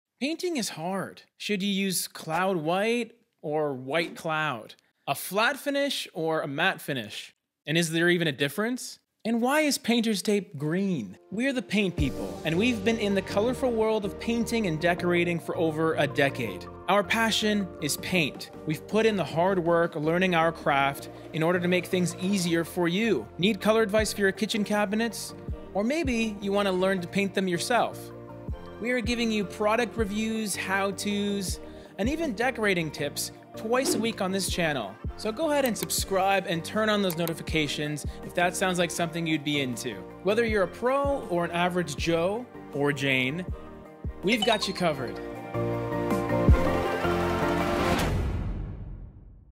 Welcome to the Paint People!
Custom Color Palettes Welcome to the Paint People!
Painting is hard. Should you use cloud white or white cloud? Flat or matte? And why is Painters tape green? We're the paint people, and we've been in the colorful world of painting and decorating for over a decade. Our passion is paint. We've put in the hard work learning our craft in order to make things easier for you! Need colour advice for your kitchen cabinets? Or maybe you want to learn to paint them yourself? We are giving you product reviews, how-to's and even decorating tips, twice a week on this channel, so subscribe and turn on those notifications if that sounds like something you'd be into. Whether you're a pro, or an average Joe (or Jane), we're the paint people, and we got you covered.